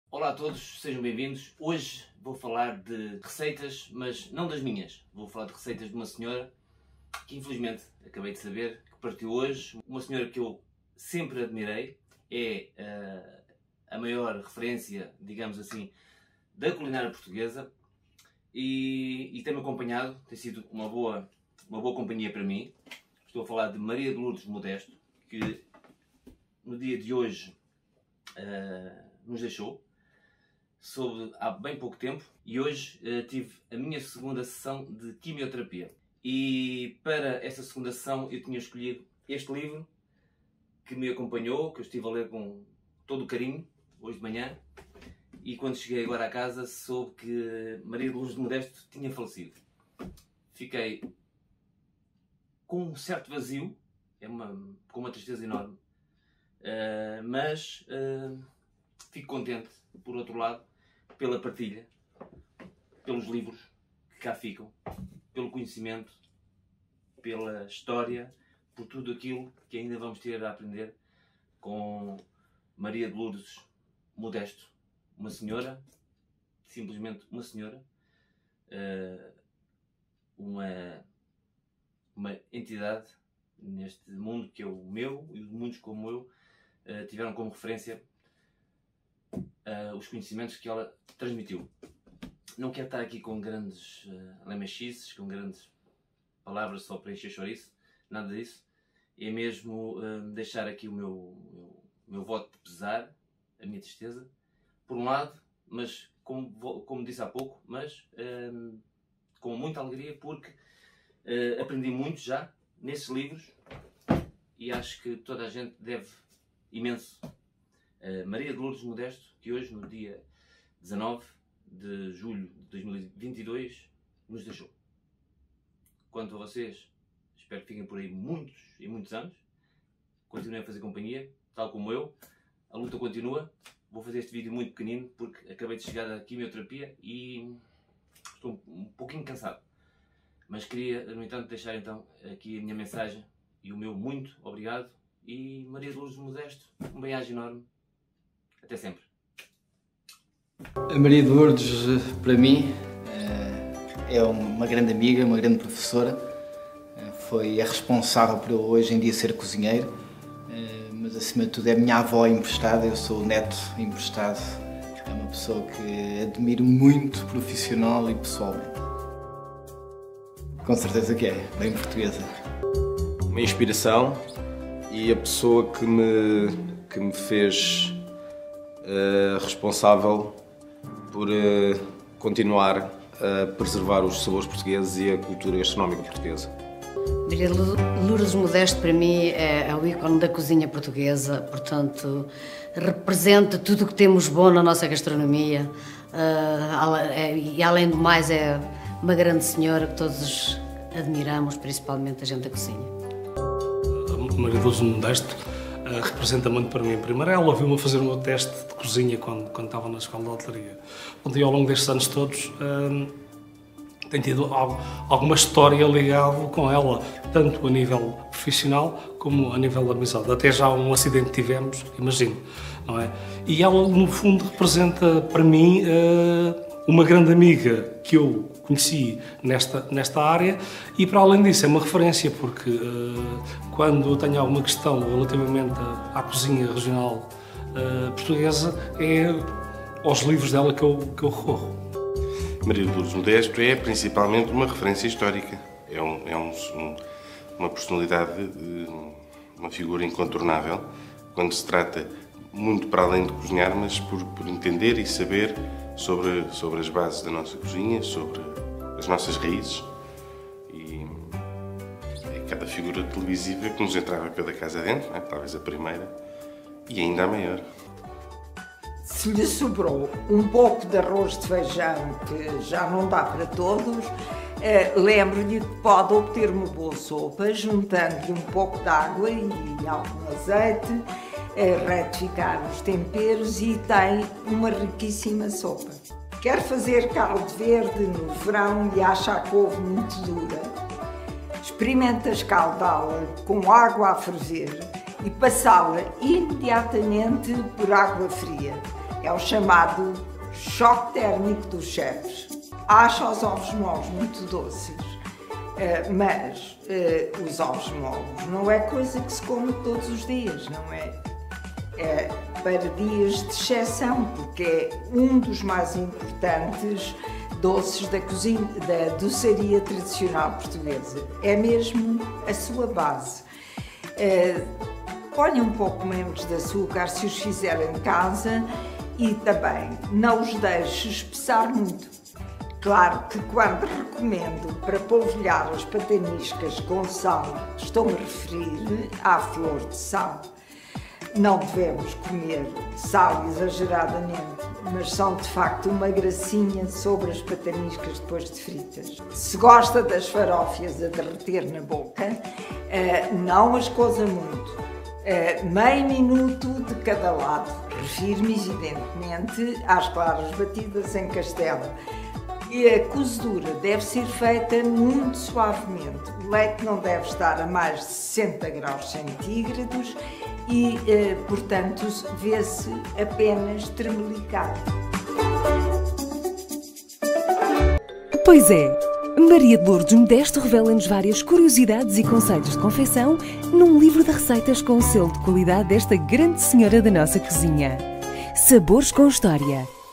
Obrigado Maria de Lourdes Modesto 1930 – 2022
Aqui podes encontrar receitas práticas para fazer em casa.

Hoje quero apenas deixar o meu agradecimento a Maria de Lourdes Modesto, que nos deixou um grande património gastronómico.

Escreva nos comentários qual receita você gostaria de ver aqui no canal !